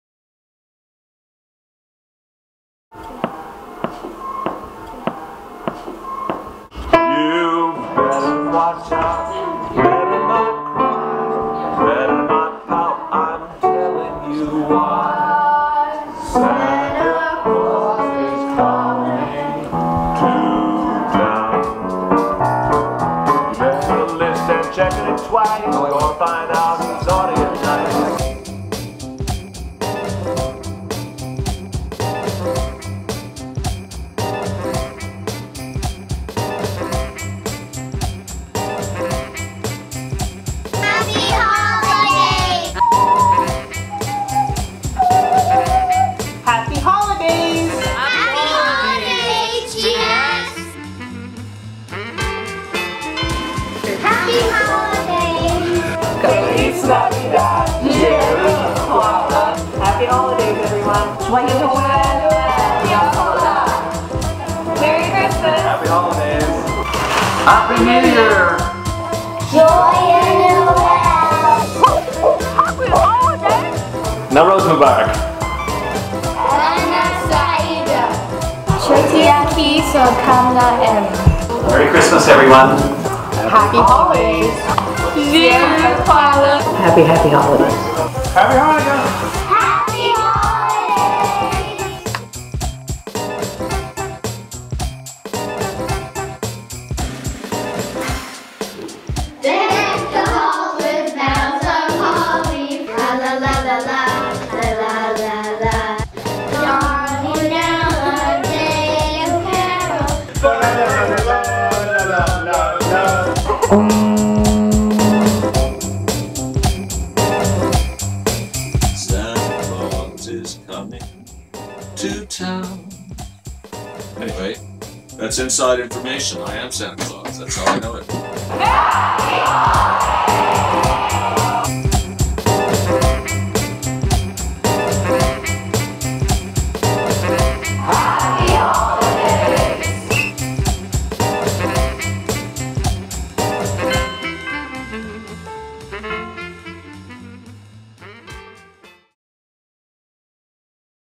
Season's Greetings from Halifax Grammar School
This video was a project of the grade nine Media Arts and Literacy students of Halifax Grammar School.